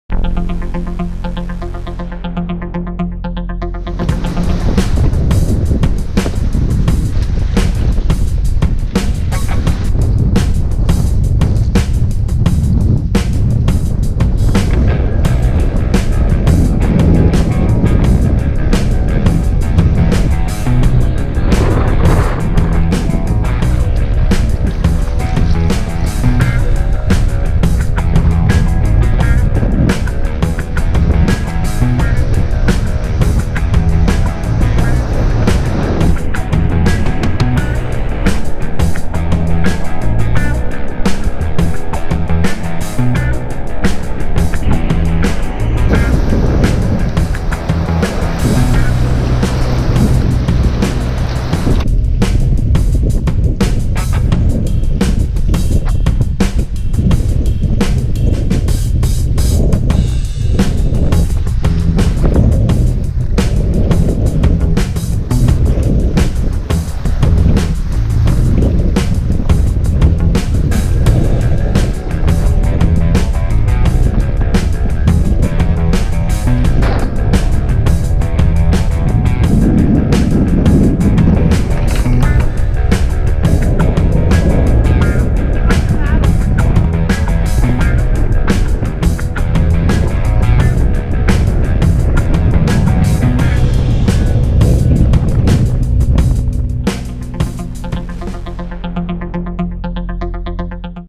Kayaking the Maumee at 33K CFS Also, New Hole at Bridge
While kayaking the Maumee river on March 29, we discovered a new hole at 33,000 cubic feet per second (CFS) at the new Waterville bridge. Take a look!